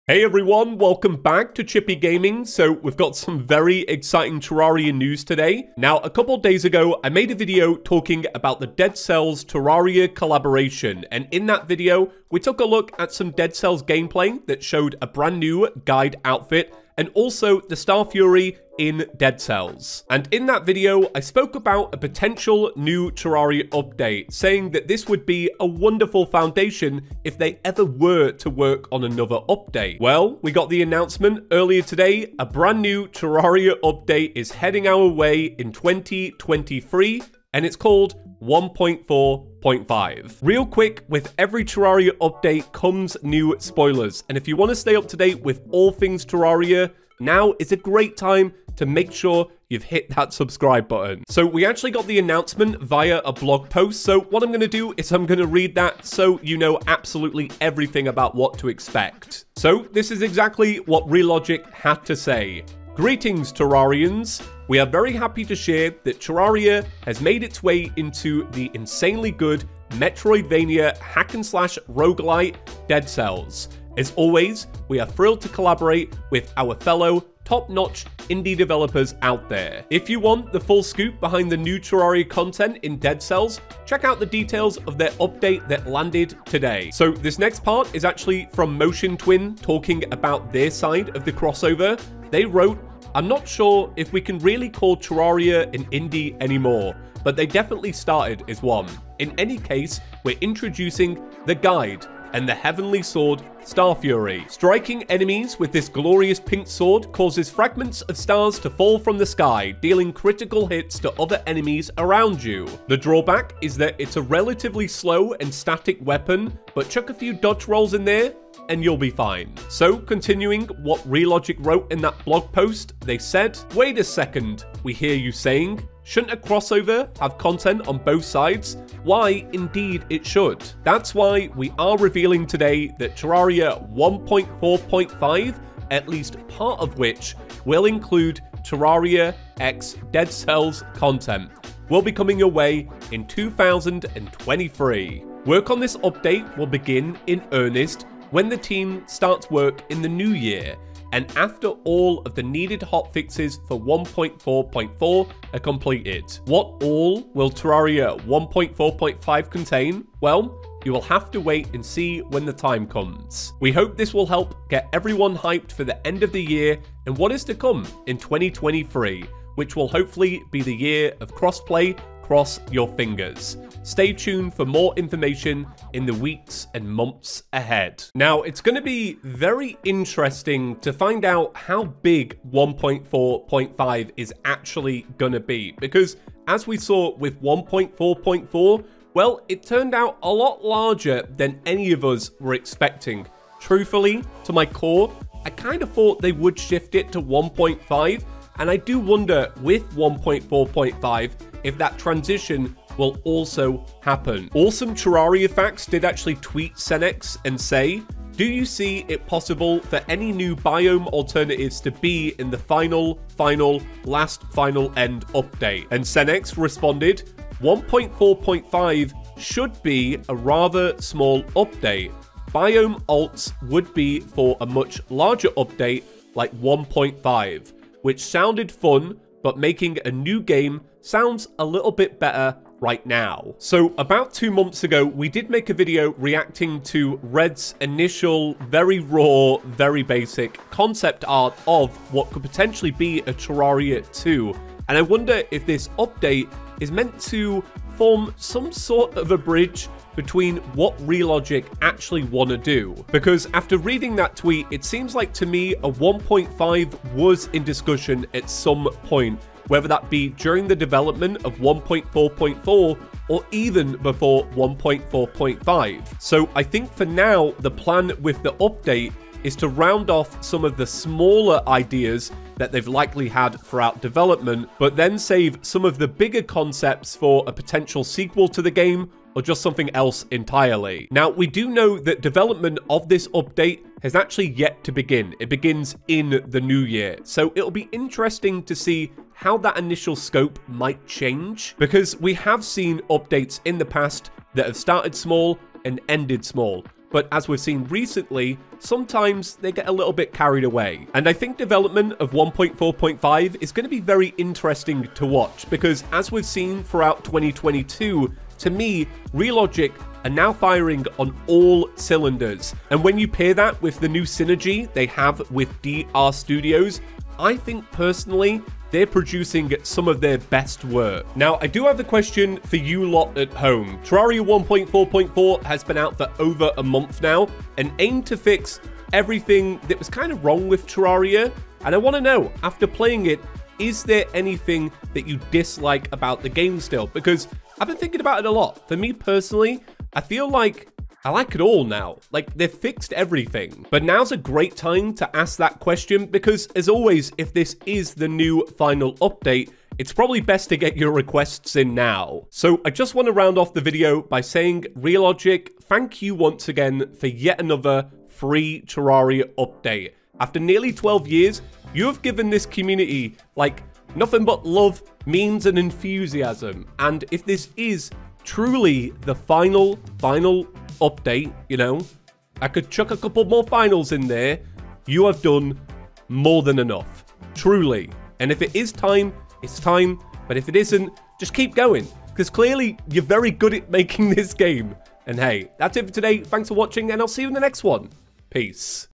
Terraria 1.4.5 Update Announcement!
In today's video, we're discussing the announcement of Terraria Update 1.4.5, which is set to release in 2023. So let's dive into the latest Terraria News & Spoilers.